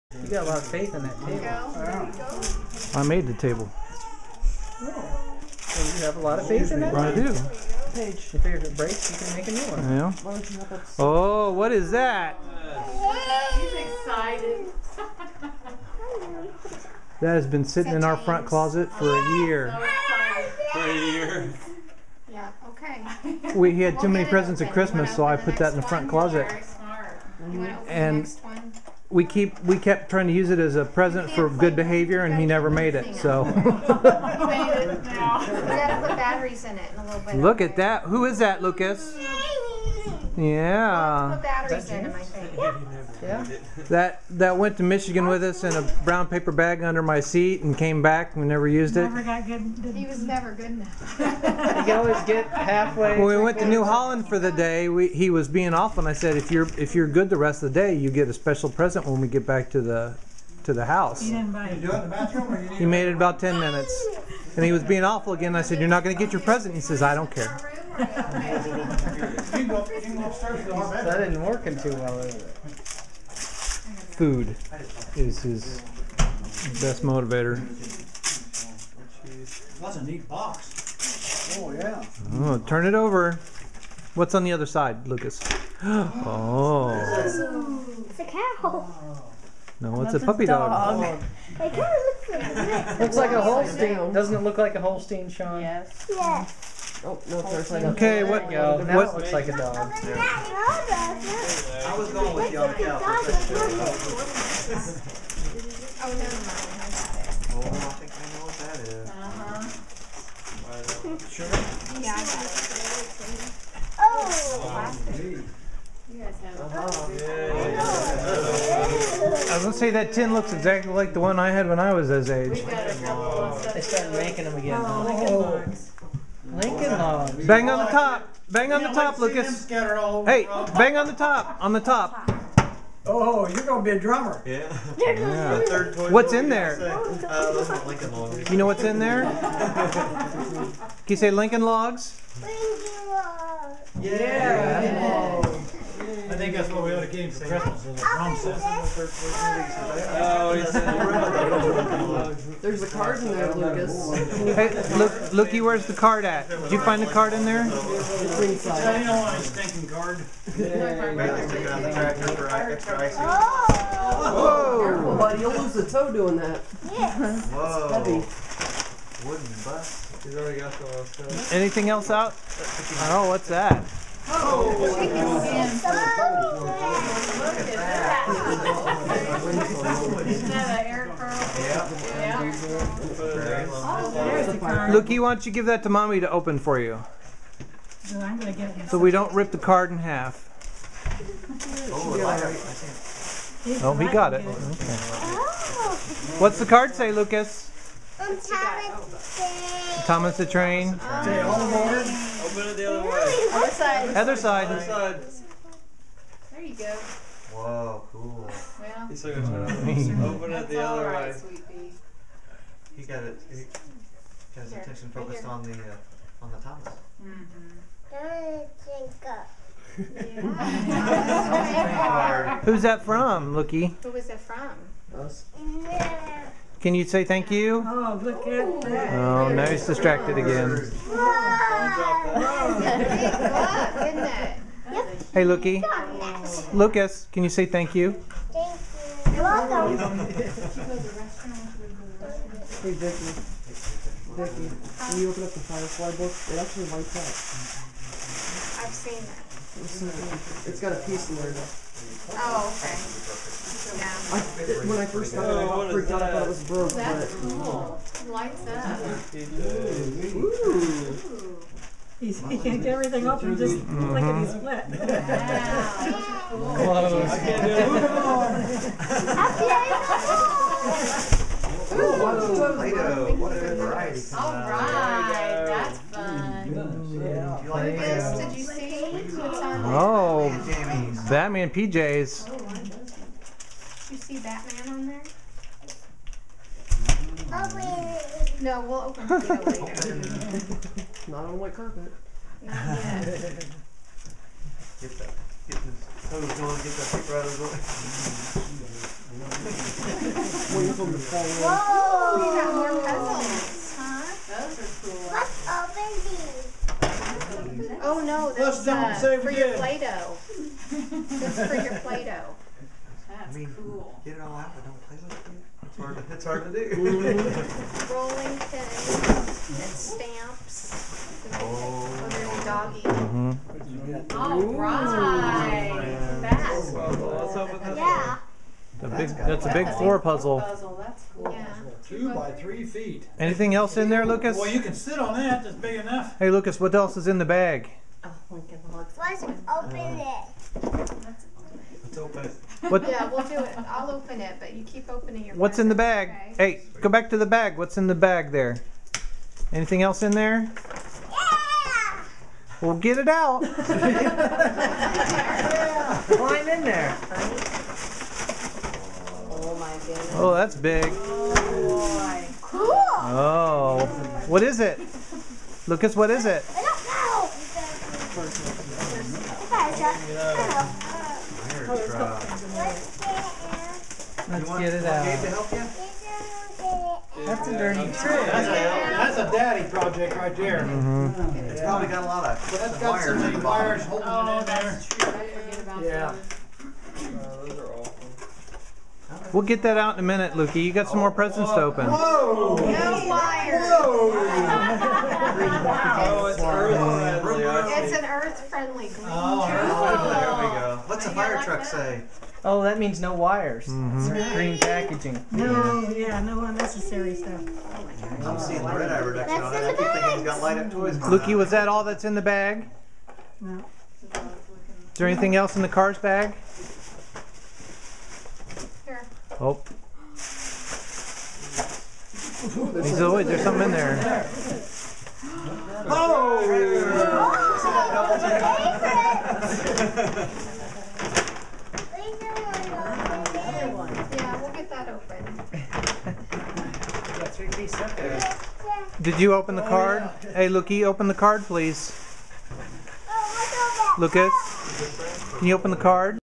Third Birthday - Presents, part 1
Lukas' third birthday party, opening presents.